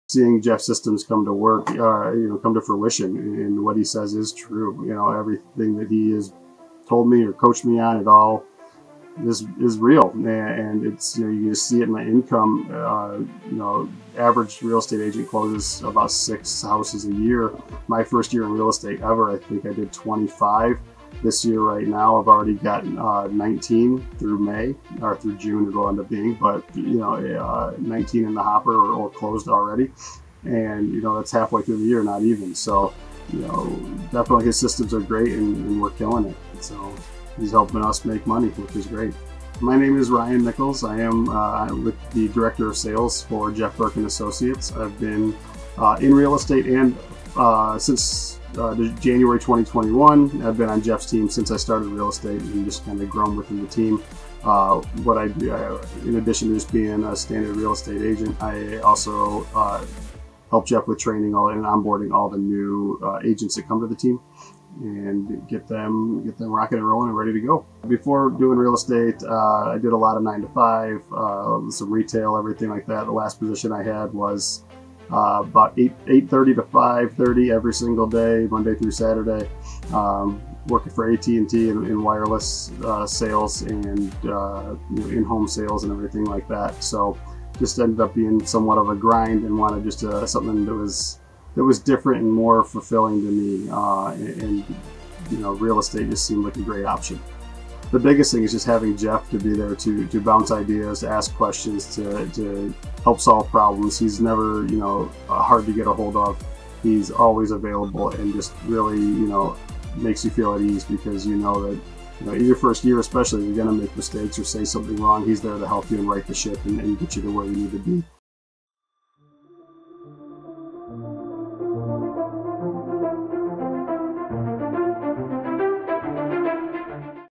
Ryan Nichols’ Review of Our Team
Here at the Jeff Burke Team, our promises are not empty, and our agent Ryan Nichols can testify to that. Ryan joined our team at the beginning of last year and has already seen crazy results following the Jeff Burke Coaching program. He previously had some unfulfilling jobs and needed a change, so he followed our coaching program and that resulted in an overabundance of closings!

Jeff Burke
The JBC’s of Real Estate
Jeff Burke & Associates
3490 Belle Chase Way Lansing MI